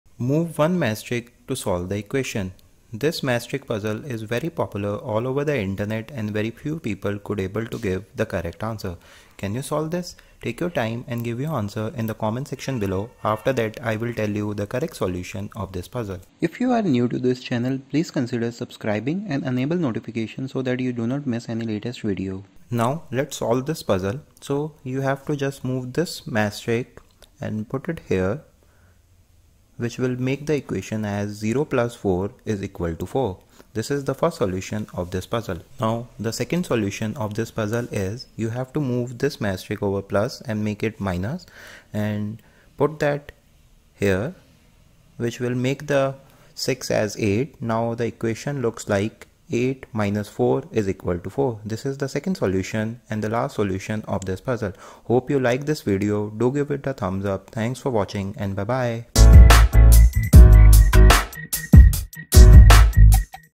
MatchStick Puzzle with answers - Move 1 Matchstick to fix it
Can you solve this matchstick puzzle 6+4=4 which is very popular all over the internet, very few people able to solve this viral matchstick puzzle. This video will show Matchstick puzzles with answers, so watch this video till the end to get the correct answer of this viral 6+4=4 matchstick riddle.

Move One matchstick to solve this matchstick riddle.

Move 1 Matchstick to fix it the maths equation 6+4=4. There can be multiple answers to this matchstick riddle.